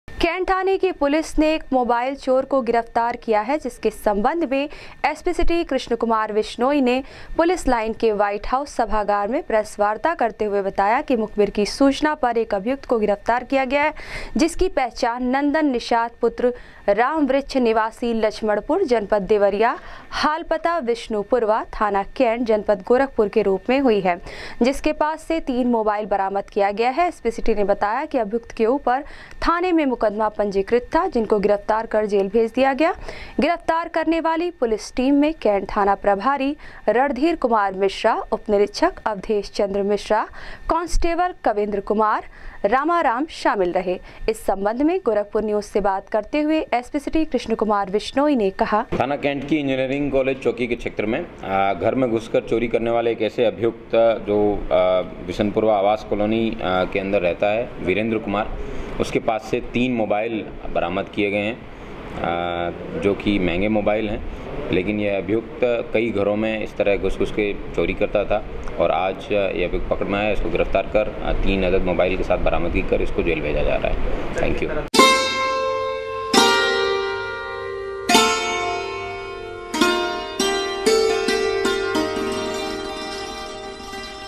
एक शातिर मोबाइल चोर को कैण्ट थाने की पुलिस ने किया गिरफ्तार,पास से तीन मोबाइल किया बरामद,एसपी सिटी
एक शातिर मोबाइल चोर को कैण्ट थाने की पुलिस ने किया गिरफ्तार,पास से तीन मोबाइल किया बरामद,एसपी सिटी ने किया खुलासा